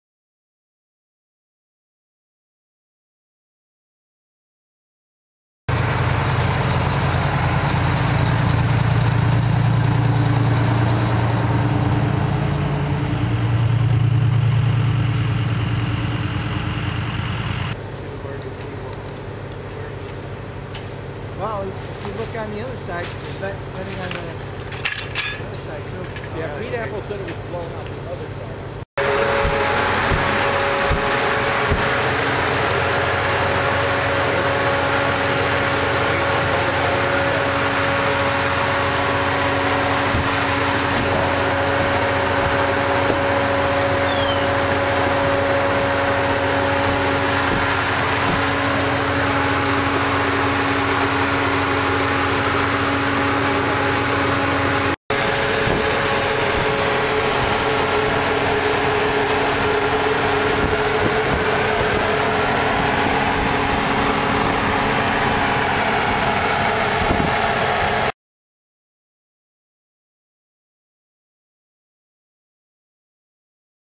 partridge st.mov
Pitttsfield, MA Firefighters respond to a housefire at 210 Partridge Road early Tuesday Morning, Feb 23, 2010.  Video by Ben Garver